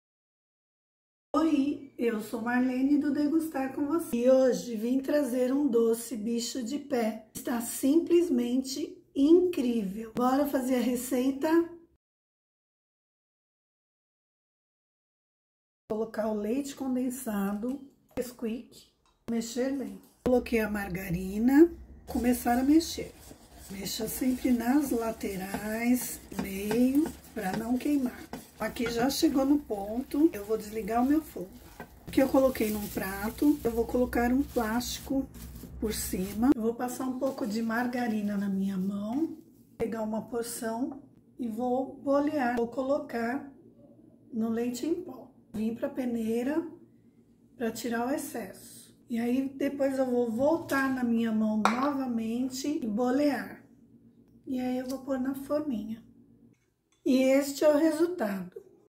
BICHO DE PÉ, O MELHOR!
Oi gente, tudo bem? Que tal uma receita fácil, muito gostosa e que as crianças vão adorar.
Veja como preparar essa incrível receita.😋

BICHO DE PÉ.
INGREDIENTES:
1 lata de leite condensado.
3 colheres (sopa) de Nesquik.
1 colher (sopa) de manteiga ou margarina.
Leite em pó.

⬇️ E comenta aqui embaixo se você tiver alguma dúvida sobre a receita, e se tiver alguma sugestão.